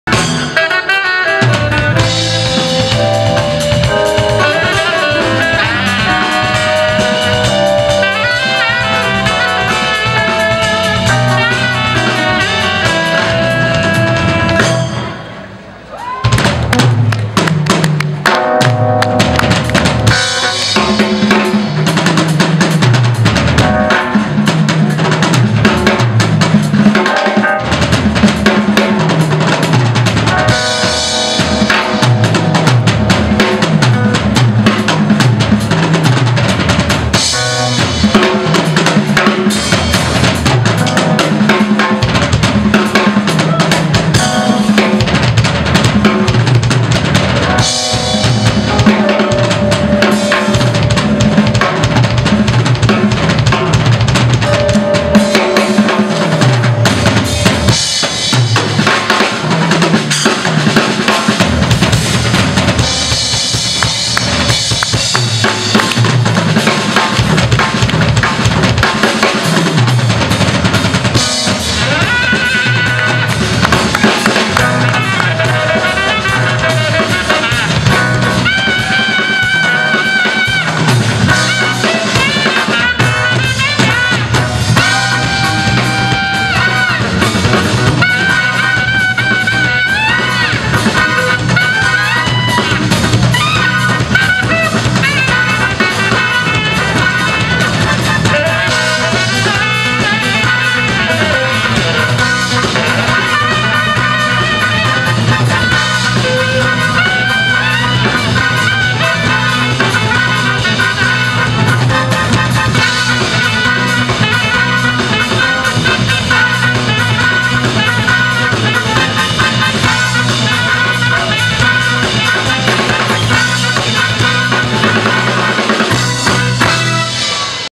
Michael Massaro featuring Steve Heathcote on drums.DSC 5151
This was the finale of Michael Massaro's (sax) set at Newmarket Jazz Festival on Sunday, August 3, 2014 featuring  Steve Heathcote.

Michael Massaro sax
Pat Kilbride  Bass
 Steve Heathcote  drums
 Marco Luciani  Keys
 Lou Bartolomucci  Guitars